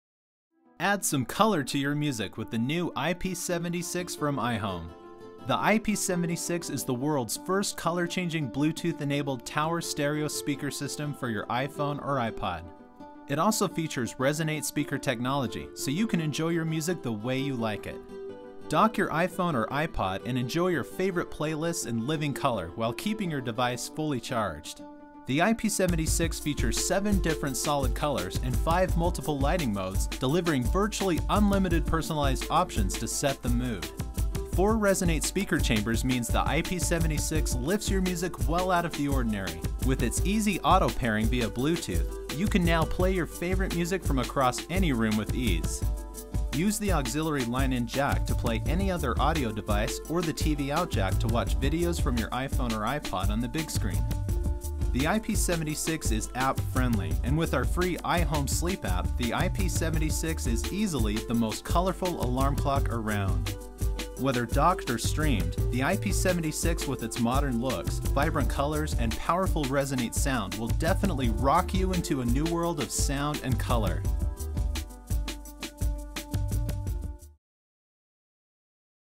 iHome iP76
Check out iHome's new iP76 Tower Stereo Speaker system! Featuring Bluetooth compatibility, color changing LEDs, Reason8 speaker chambers, aux line in and video out jacks!